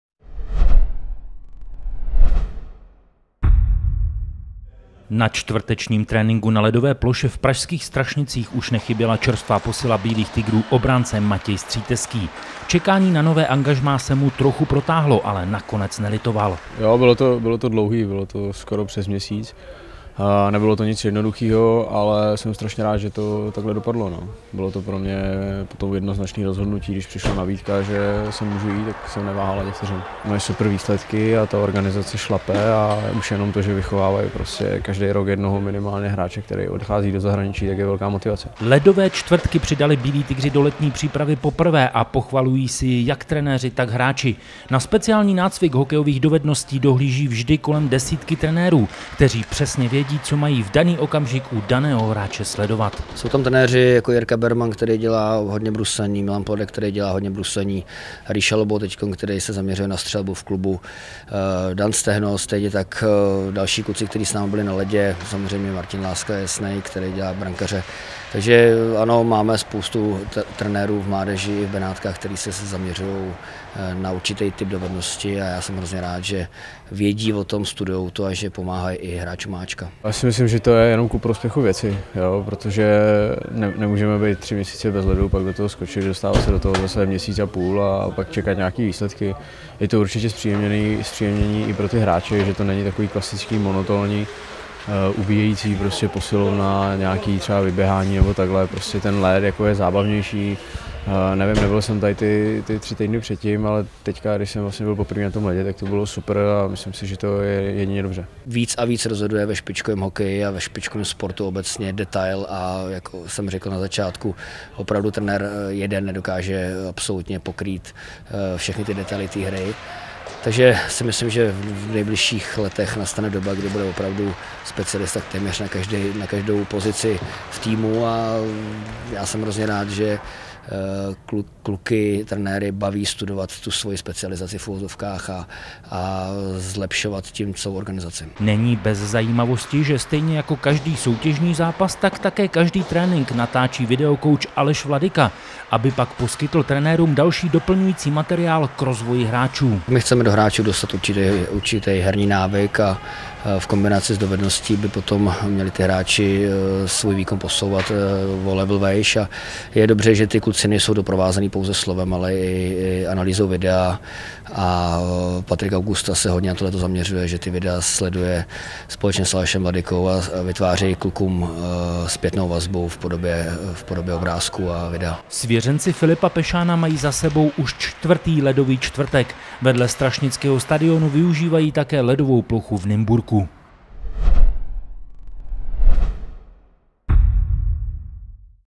Filip Pešán: Hokej rozhodují detaily
Ledové čtvrtky přidali Bílí Tygři do letní přípravy poprvé a pochvalují si je jak trenéři, tak hráči.